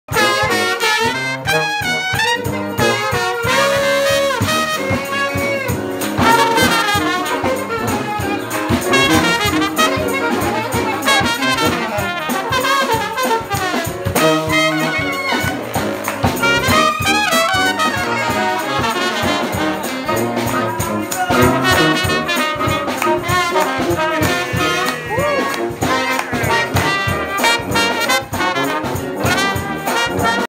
Alphonso Horne and the Gotham Kings tearing it up
Ginny's Supper Club in Harlem.